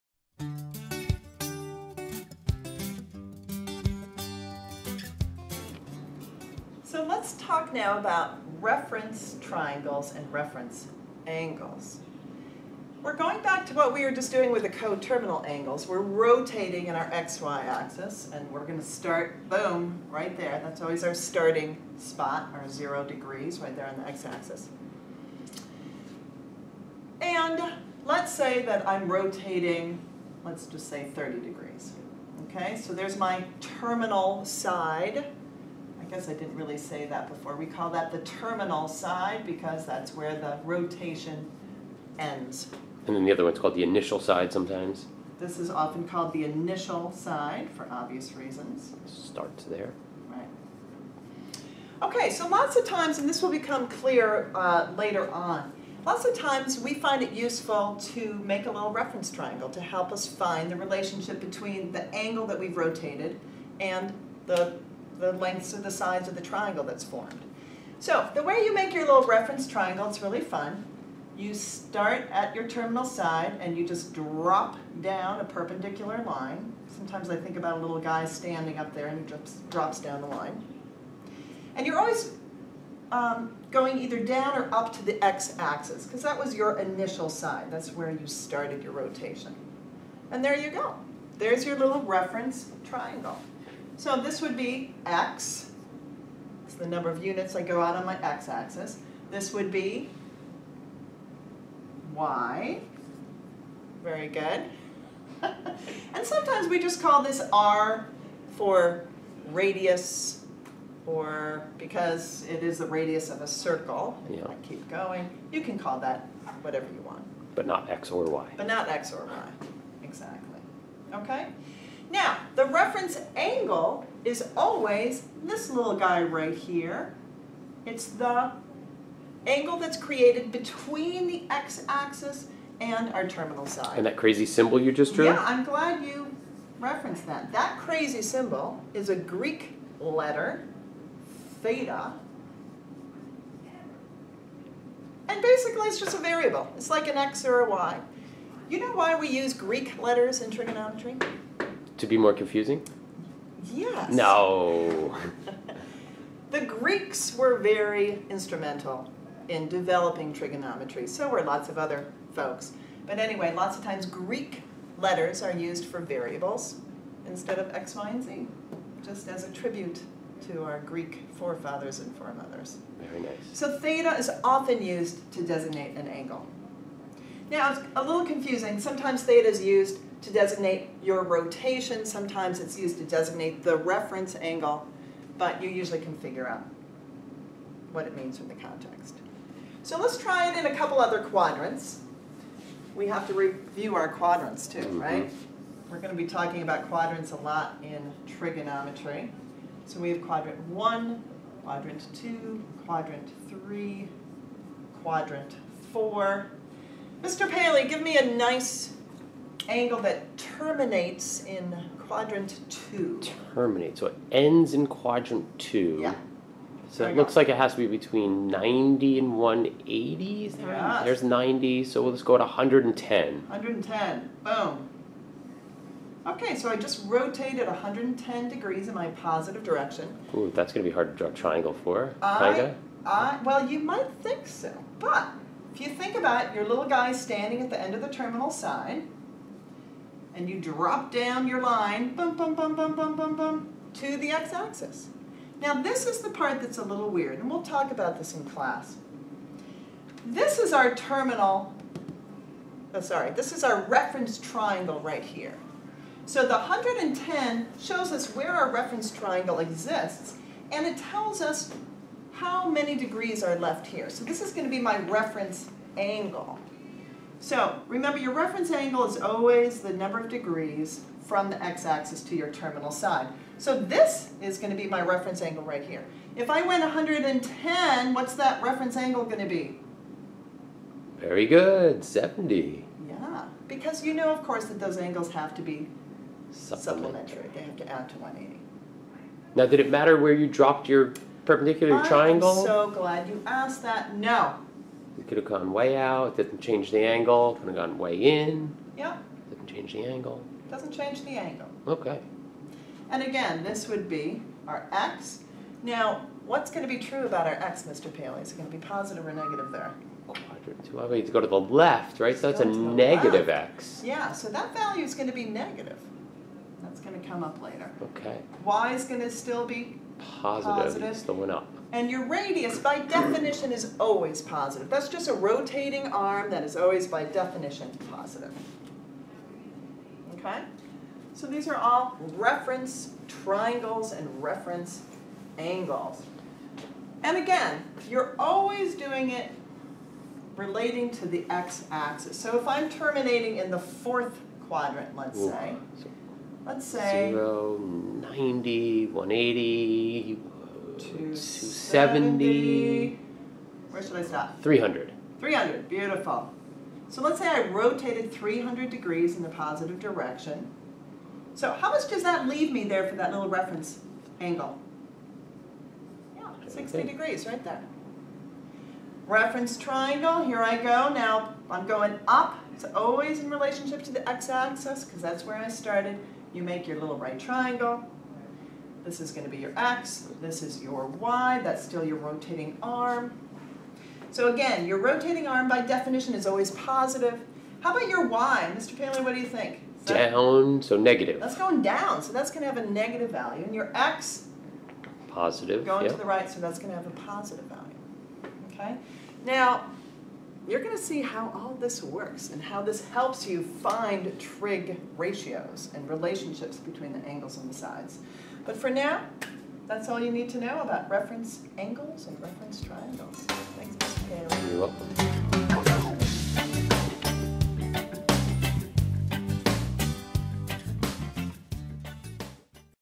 trigonometry reference angles and triangles
Introduction to reference angles and reference triangles in trigonometry